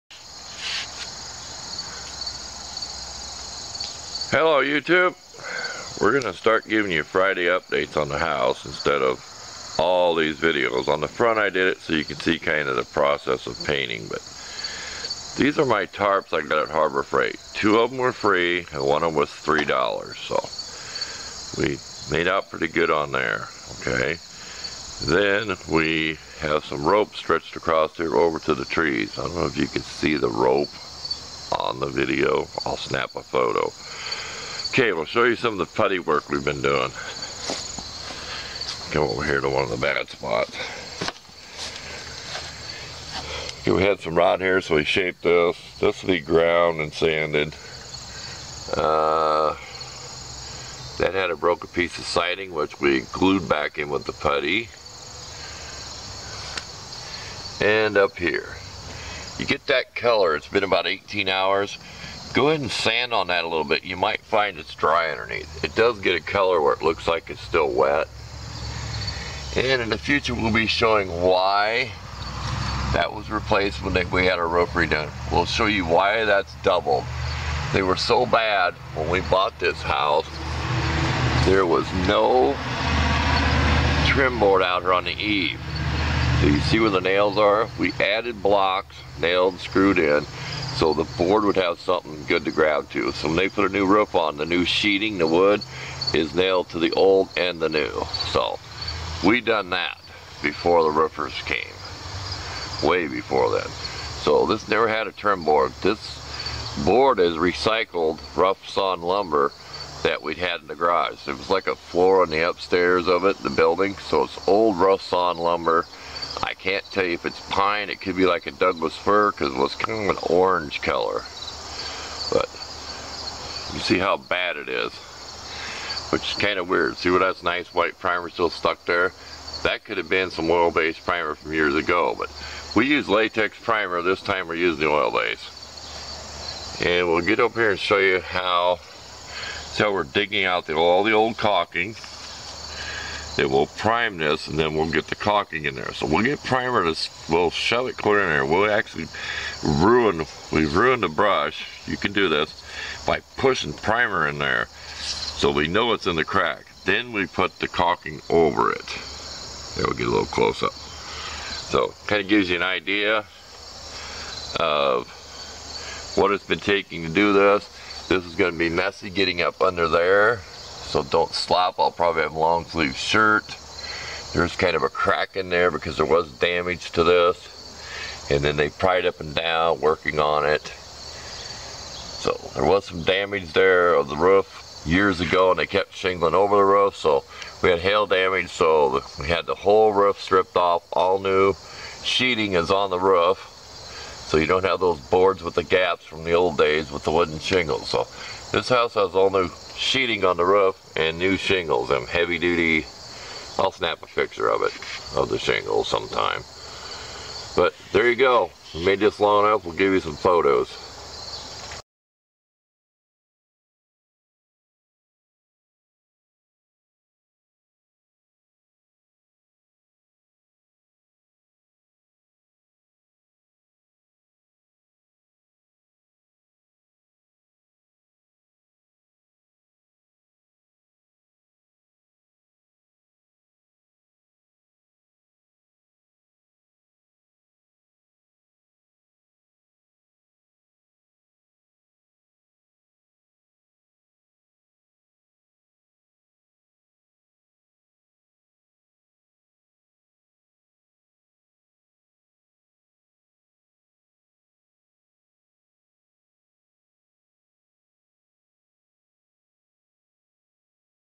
Update on the House Painting 1
We will be posting an update once a week on the house painting ...We put up tarps for shade and in case it rains while painting